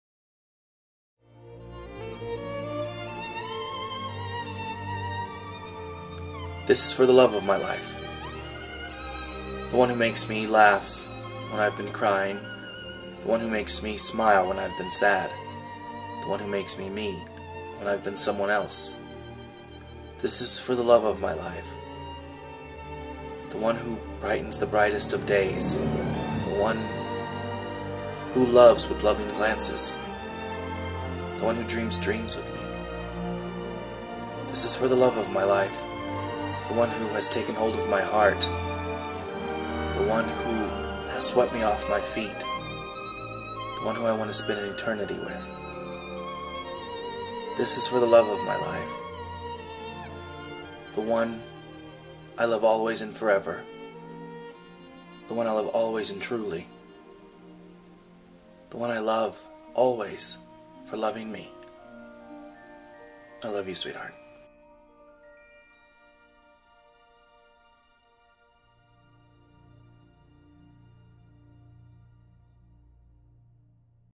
A Love Poem
I wrote a love poem for my sweetheart. Yes I know it's cheesy (candle and all), but I really do mean every word in the poem.